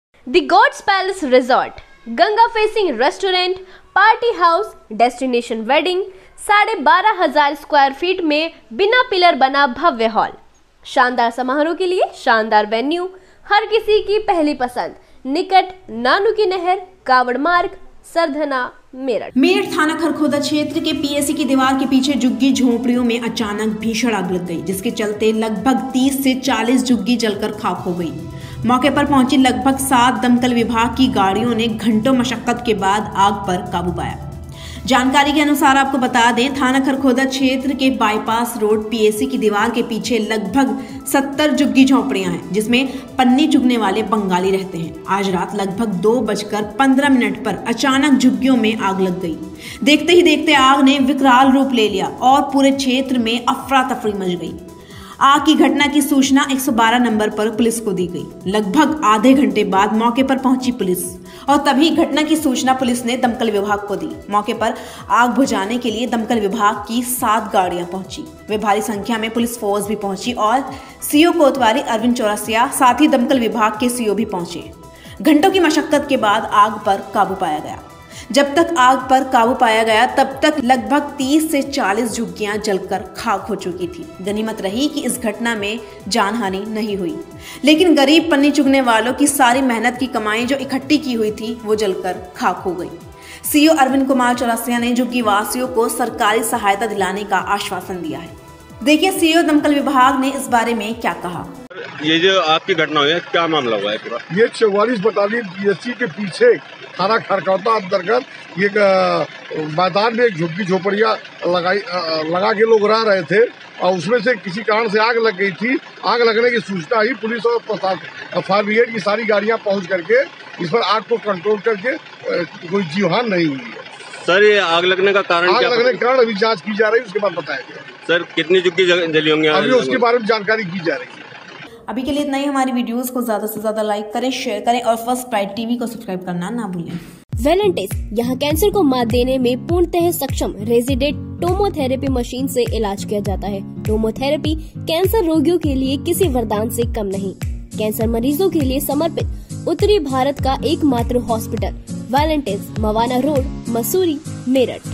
हापुड़ रोड पर भीषण आग, 40 से ज्यादा झुग्गी झोपड़ी हुई राख First byte.tv
झुग्गी– झोपड़ियों में लगी भीषण आग

थाना खरखौदा क्षेत्र की पीएसी दीवार के पीछे झोपड़ियों में लगी आग

30 से 40 झोपड़िया जलकर हुई खाक

मौके पर पहुंची 7 दमकल विभाग की टीम ने पाया आग पर काबू

सीओ अरविंद चौरसिया ने दिया सरकारी सहायता का आश्वासन